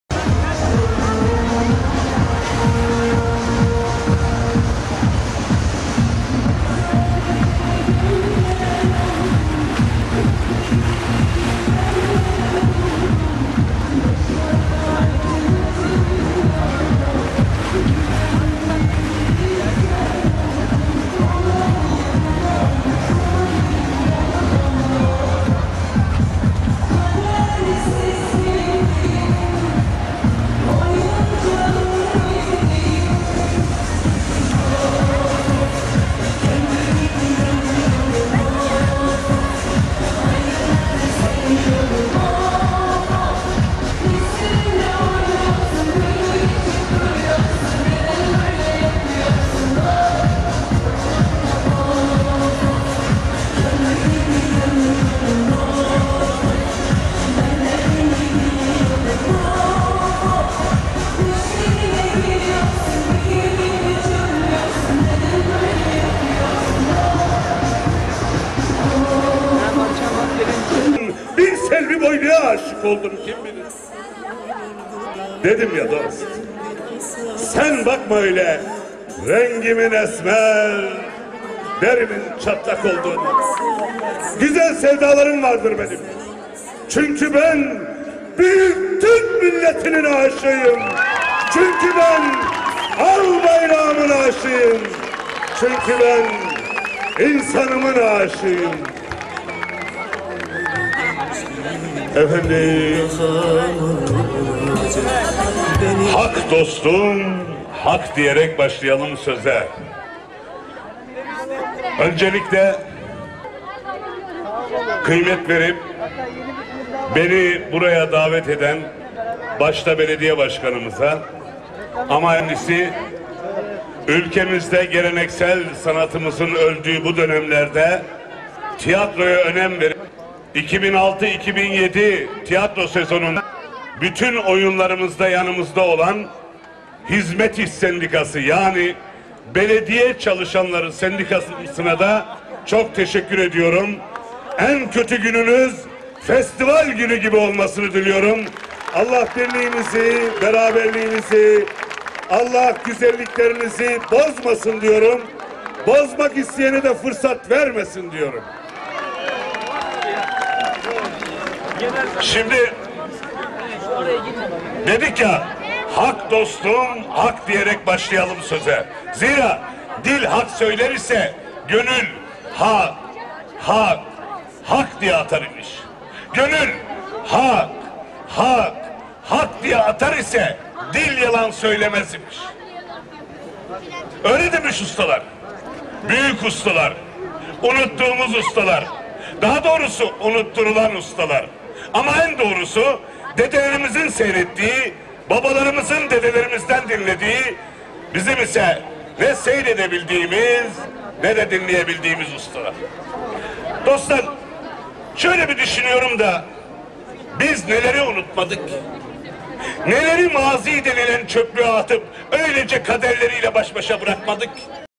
2006 da Ahmet Yenilmez Ayancık'taydı
28 Temmuz 2006 Festivalde Ahmet Yenilmez Ayancıktaydı